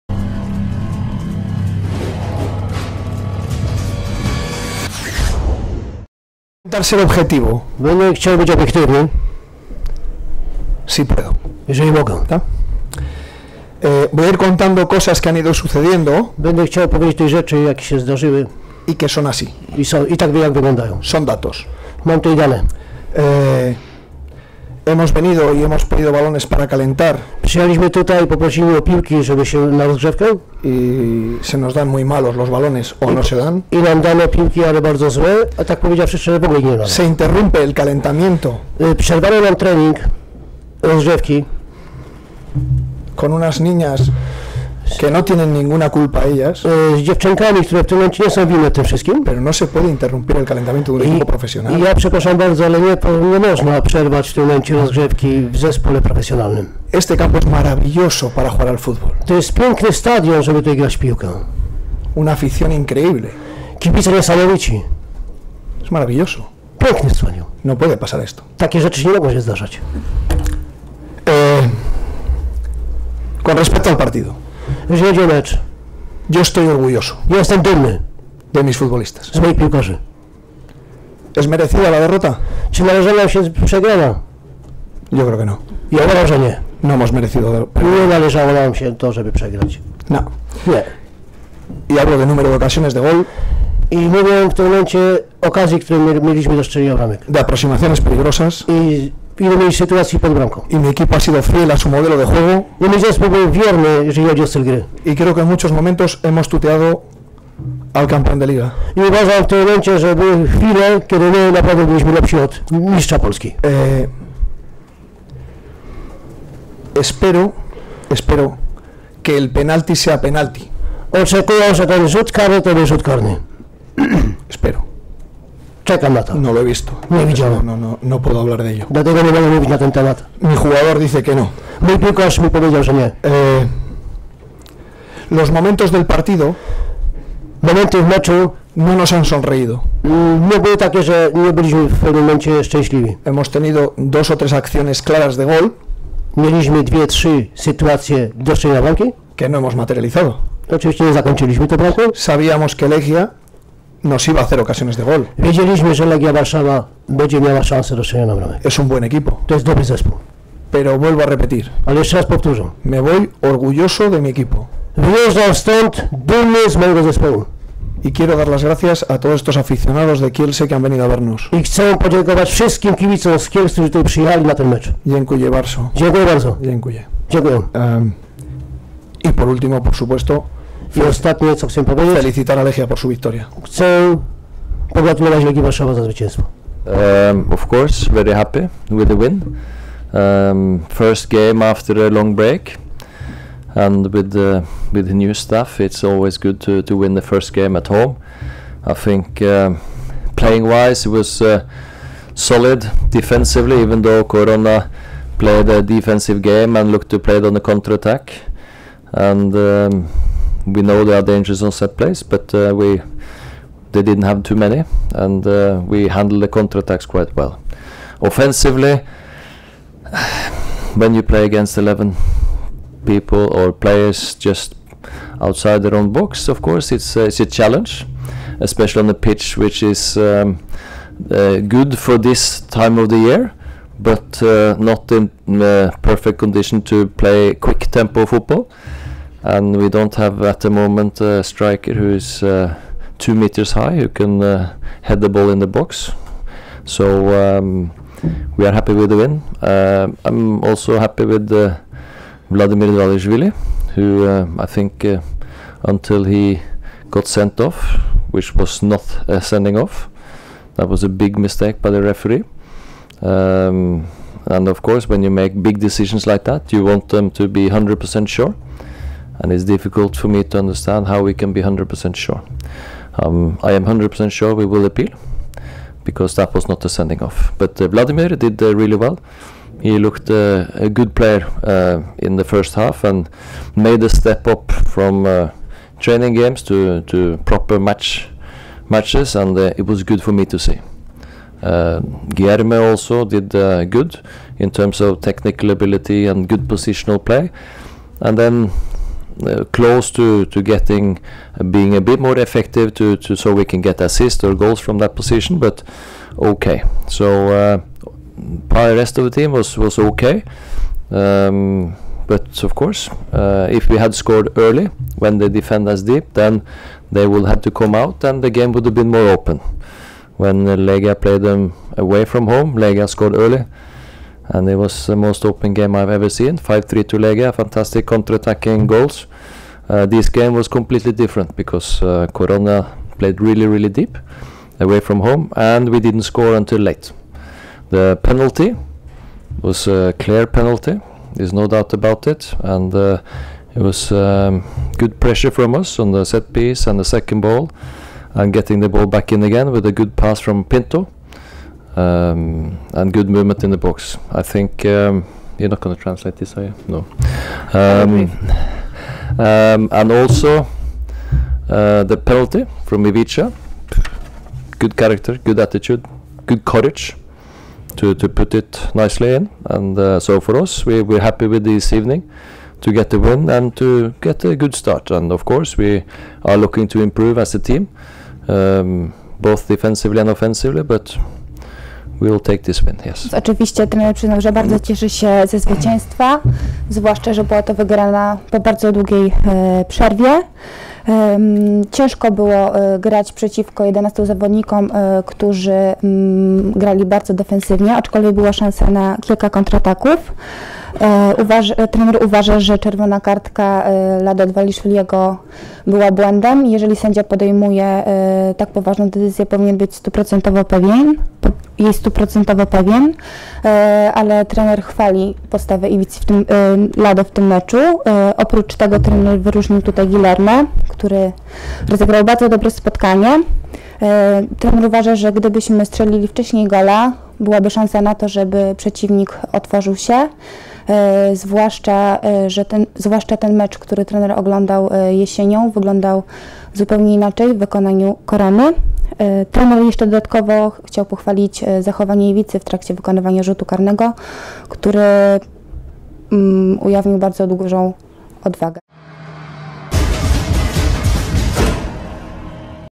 Konferencja po meczu Legia Warszawa - Korona Kielce 1:0 (14.02 2014 r.)
- Jestem dumny z moich piłkarzy. Nie należała nam się przegrana. Mieliśmy wiele sytuacji. Zespół był wierny stylowi, który sobie założyliśmy. Były nawet chwile, gdy byliśmy lepsi od mistrza Polski -- mówił rozgoryczony Jose Rojo Martin po przegranej z Legią Warszawa.